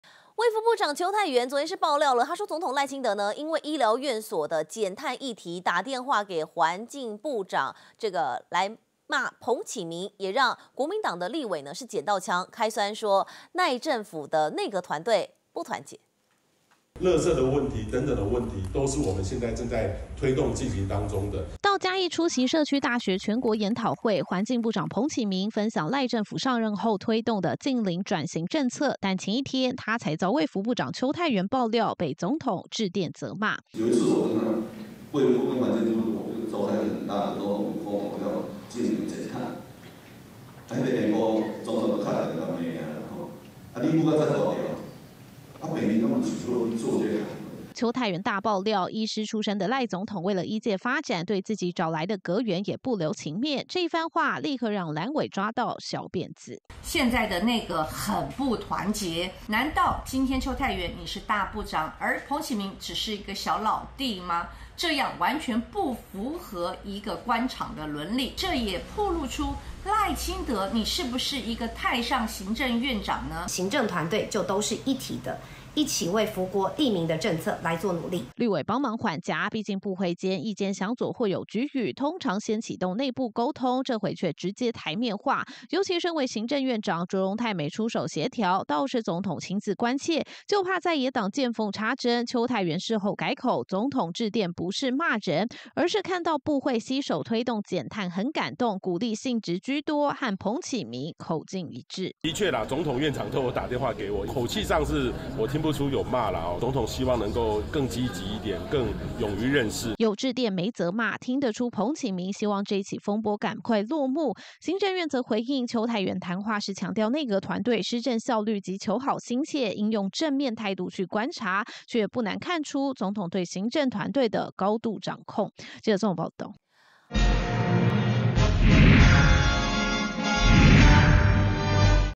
網內互打？ 邱泰源爆「總統打電話罵環境部長」
衛福部長邱泰源昨（21）日爆料，總統賴清德因醫療院所減碳議題致電罵環境部長彭啓明，讓國民黨立委撿到槍，開酸賴政府內閣團隊不團結。邱泰源事後改口，說自己與彭啓明是好朋友雙方溝通無礙。彭啓明今（22）日受訪時證實總統有致電關心，但口氣並無責罵，而是希望他能更積極、更勇於任事。

下載【年代電視】APP：
iOS平台：